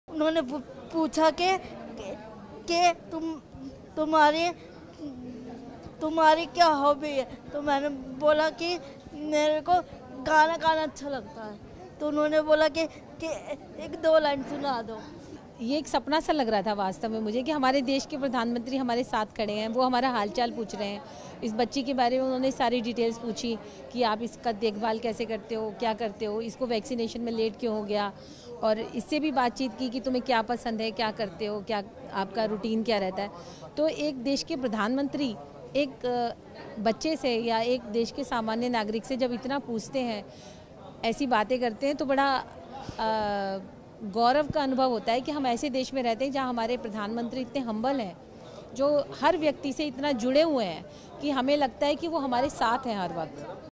Our Prime Minister is so humble, says mother of Divyang child, who met PM Modi at RML Hospital
Prime Minister Narendra Modi interacted with healthcare workers and COVID-19 vaccine beneficiaries at RML Hospital in Delhi as the cumulative vaccine doses administered in the country surpassed the landmark 100-crore mark.